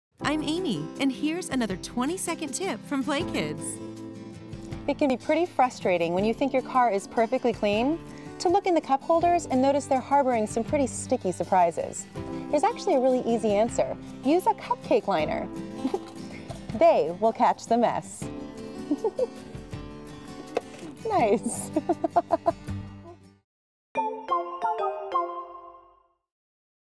20 Sec Tips: Save The Cup Holders | DYI Hacks for Parents
We can all relate to the feeling of frustration that comes right after thinking your done cleaning the car, only to find the cup holders are harboring some gross and sticky surprises. Watch this “20 Second Tip” from PlayKids to learn a little secret that will keep your cupholder looking brand new!

Do you want more fun and educational contents for kids?